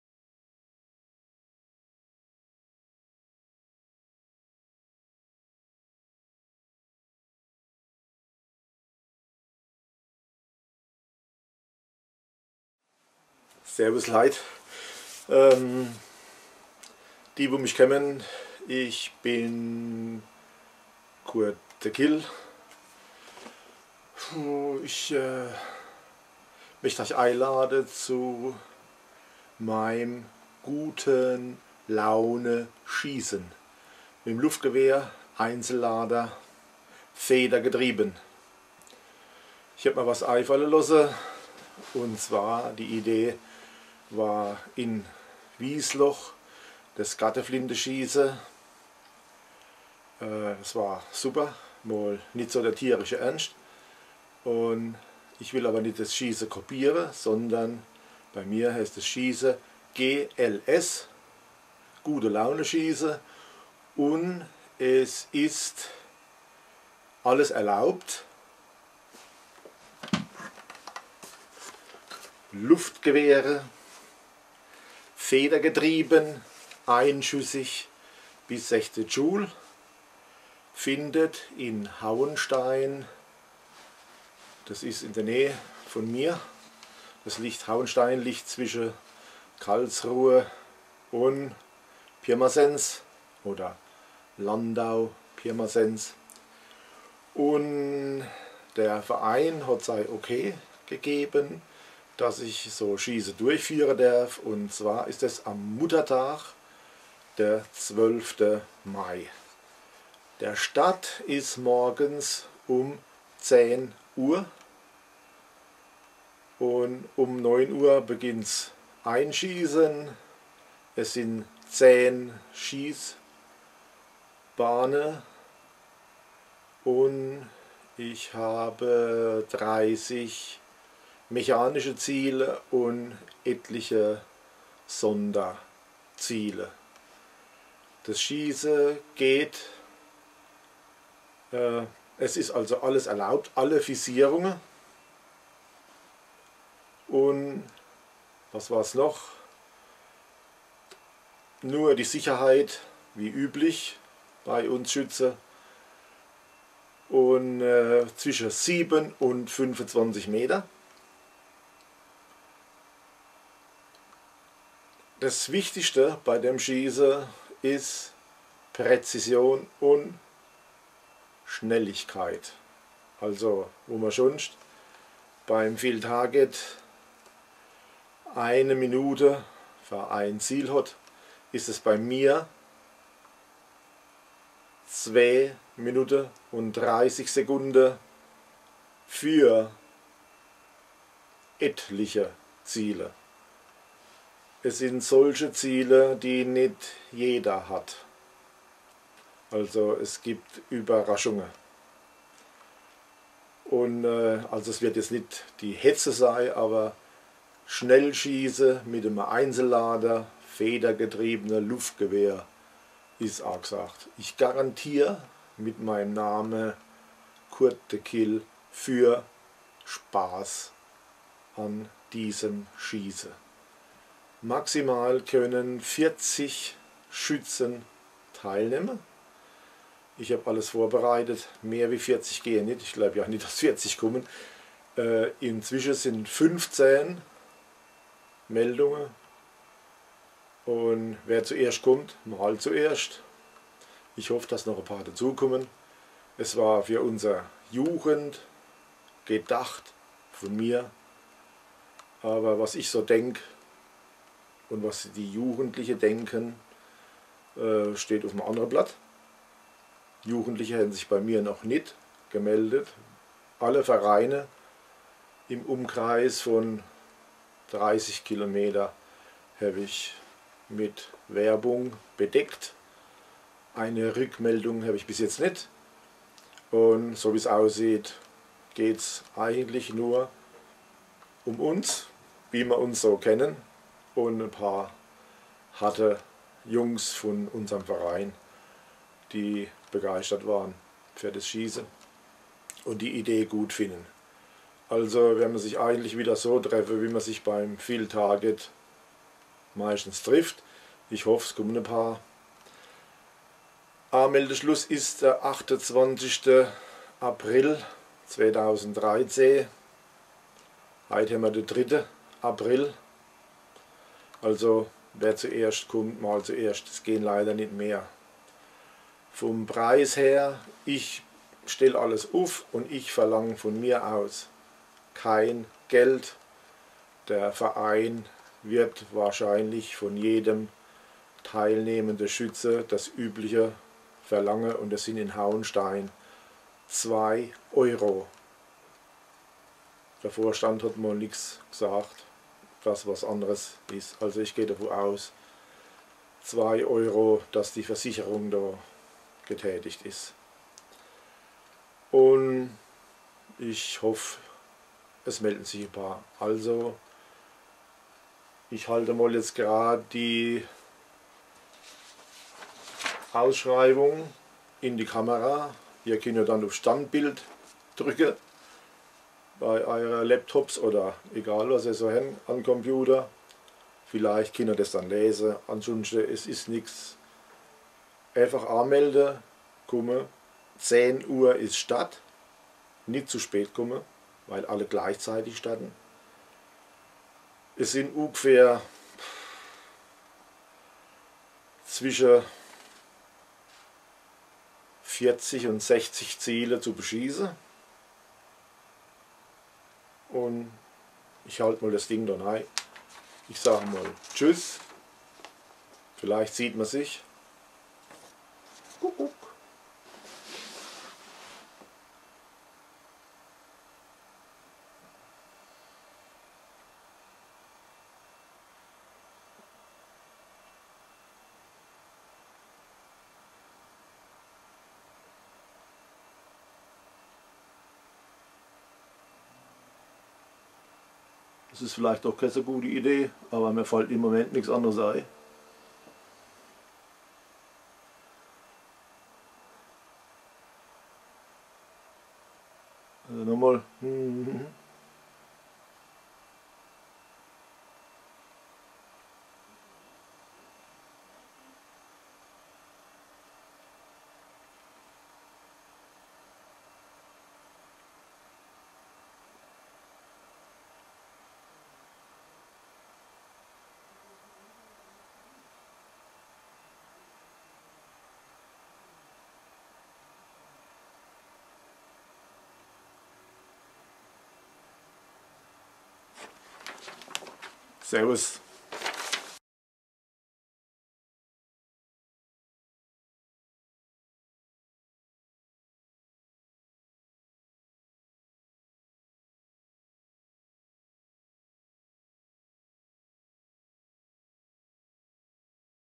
Field Target - G.L.S. SCHIEßEN bei Kurt "de Kill" www. pfalzlinks.de
Field Target G.L.S. schießen bei Kurt "de Kill" in der Pfalz.
Austragungsort Schützenverein St. Georg in Hauenstein.
Max. 40 Teilnehmer
Anmeldung bis 28. April 2013